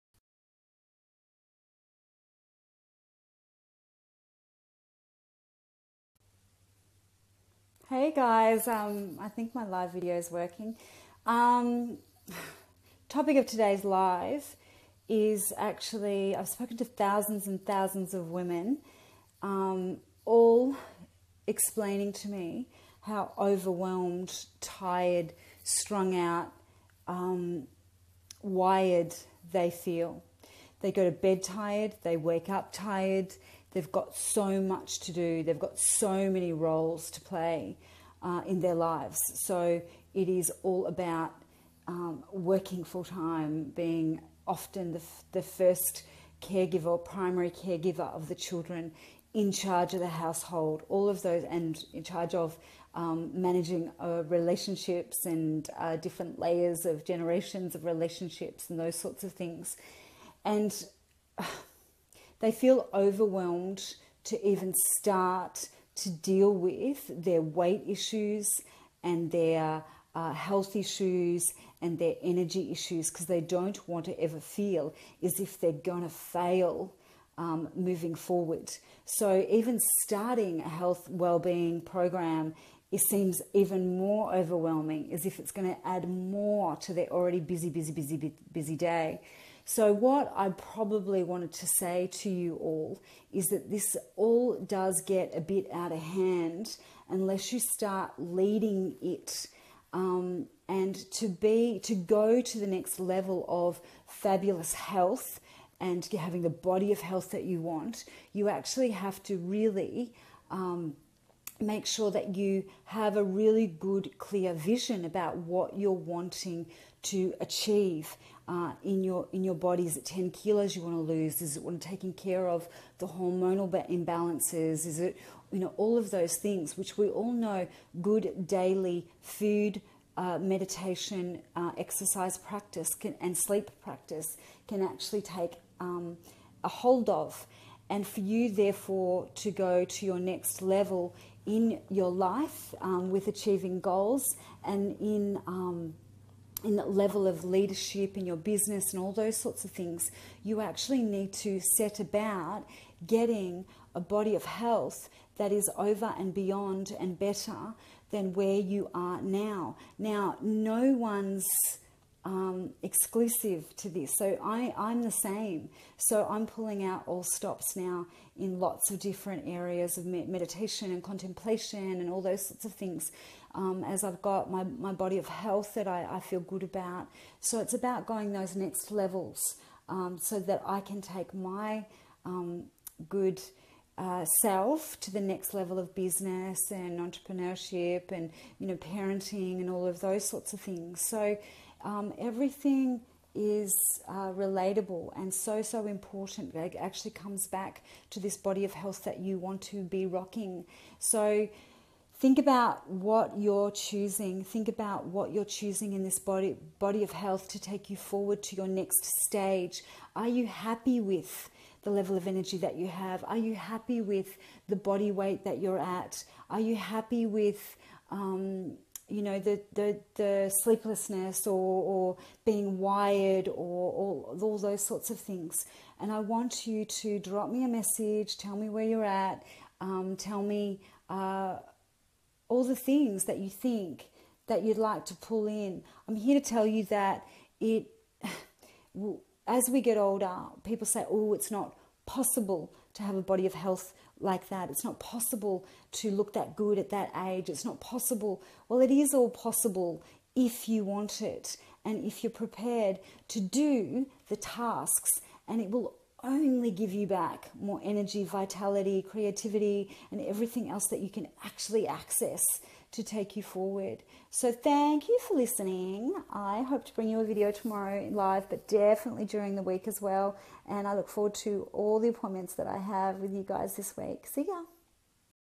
Tired, sick, run down ? I have spoken to 1000's of women not coping
WHAT to do next if tis is you. Feeling over weight, tired and depressed.